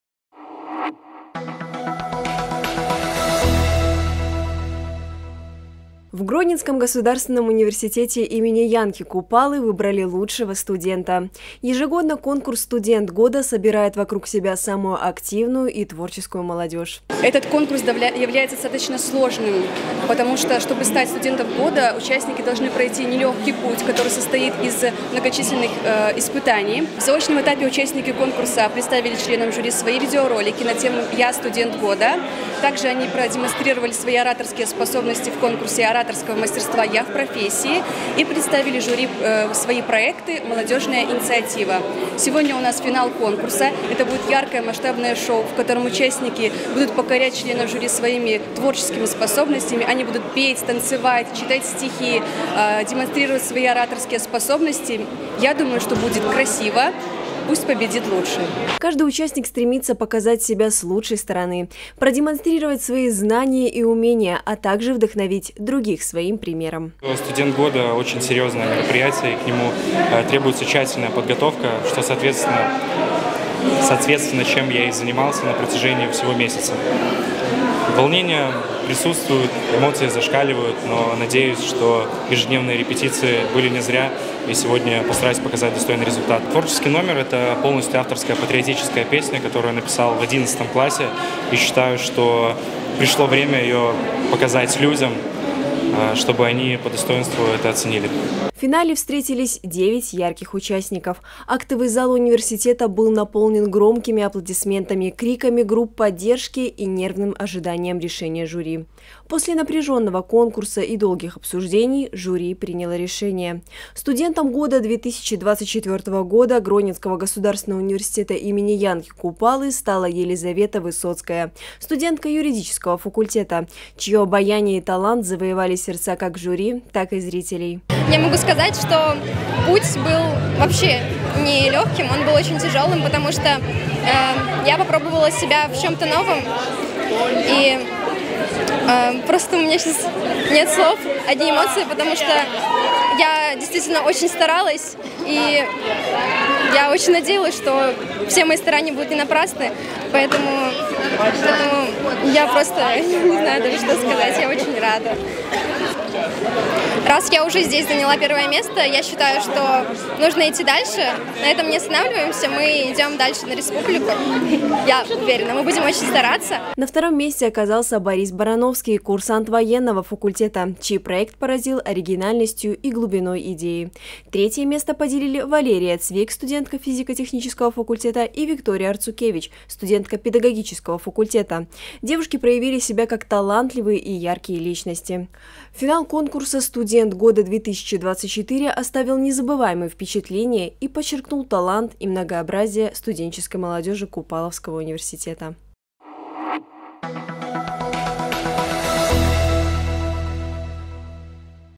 В ГрГУ имени Янки Купалы состоялся конкурс «Студент года – 2024»
31 октября в ГрГУ имени Янки Купалы состоялся конкурс «Студент года – 2024». В финале встретились девять ярких участников, готовых продемонстрировать свои таланты и страсть к искусству. Актовый зал университета был наполнен громкими аплодисментами, криками групп поддержки и нервным ожиданием решения жюри.

«Студентом года – 2024» ГрГУ имени Янки Купалы стала Елизавета Высоцкая, студентка юридического факультета, чье обаяние и талант завоевали сердца как жюри, так и зрителей.

На втором месте оказался Борис Барановский, курсант военного факультета, чей проект поразил оригинальностью и глубиной идей.

Третье место поделили Валерия Цвик, студентка физико-технического факультета, и Виктория Арцукевич, студентка педагогического факультета, проявившие себя как талантливые и яркие личности.

Победители номинаций конкурса заслужили особое внимание. Лучший проект – Борис Барановский, курсант военного факультета, чей творческий подход идеально сочетался с профессионализмом. Лучший оратор – Валерия Цвик, студентка физико-технического факультета, умело владеющая словом и способная вдохновить любую аудиторию. Лучший видеоролик – Елизавета Высоцкая, студентка юридического факультета, чье видео впечатлило жюри и зрителей своей глубиной и креативностью. Лучший творческий номер – Валерия Цвик, студентка физико-технического факультета, удивившая всех оригинальным и захватывающим выступлением. Выдающийся интеллект – Виктория Арцукевич, студентка педагогического факультета, чьи знания и аналитические способности вызвали восхищение жюри. Лучшая визитная карточка – Ксения Яроцевич, студентка факультета искусств и дизайна, чья креативность и оригинальный подход поразили всех присутствующих.